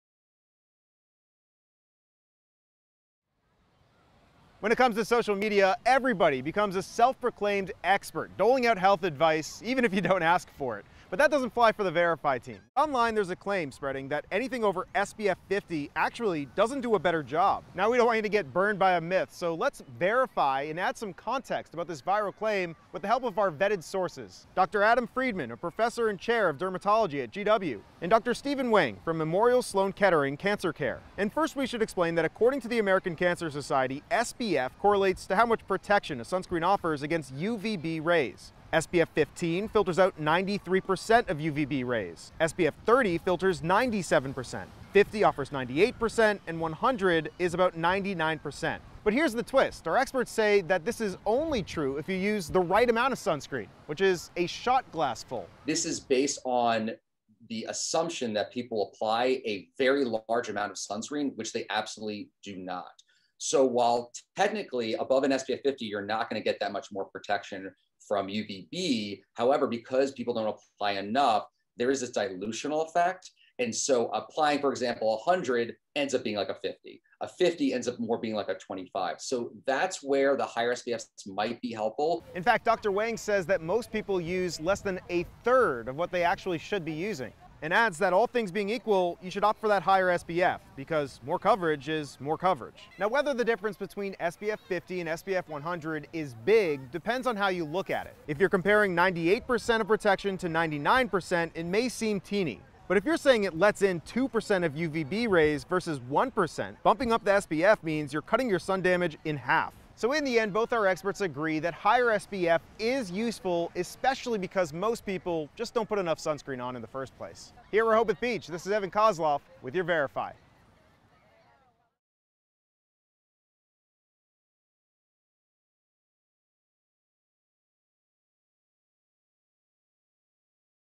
VERIFY: Are sunscreens higher than SPF 50 any better?
As summer heat hits its peaks, more people are headed out to the beach or the pool to bask in the sun.
And while everyone should be using sunscreen to protect from the sun's potentially harmful rays, sometimes there's a dispute over whether going above an SPF 50 is really that much more beneficial; So we're verifying.